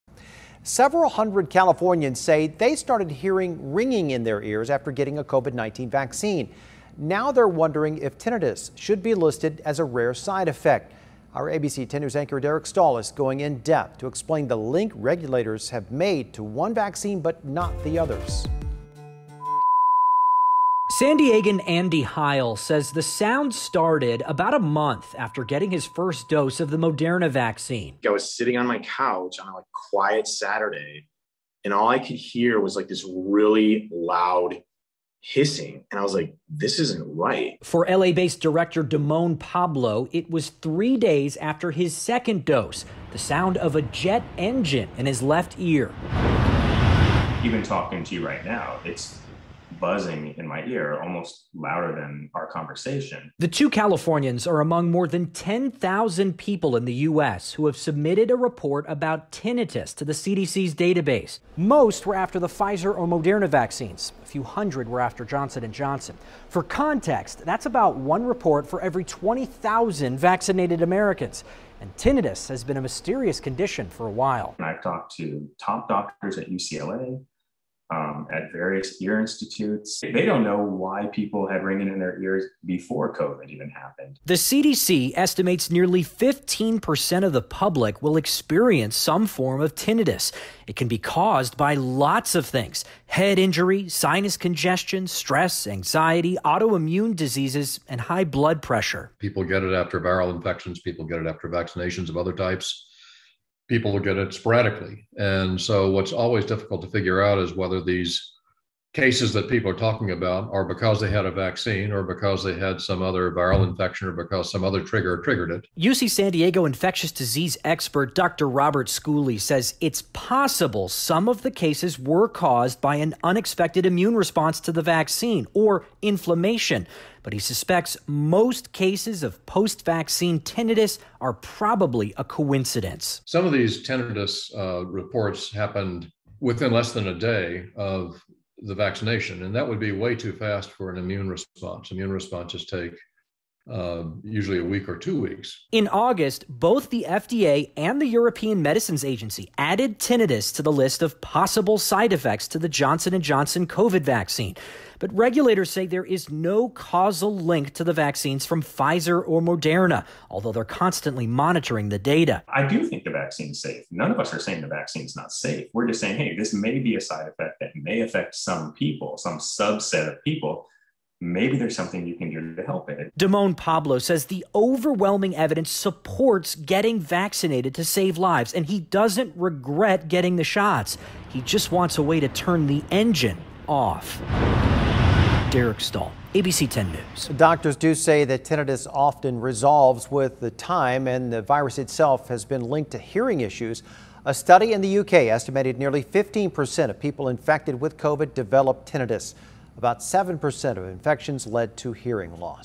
In-Depth: Can the COVID-19 vaccines cause ringing in the ears or tinnitus?
The FDA added tinnitus as a potential side effect to one COVID vaccine. But what about the others?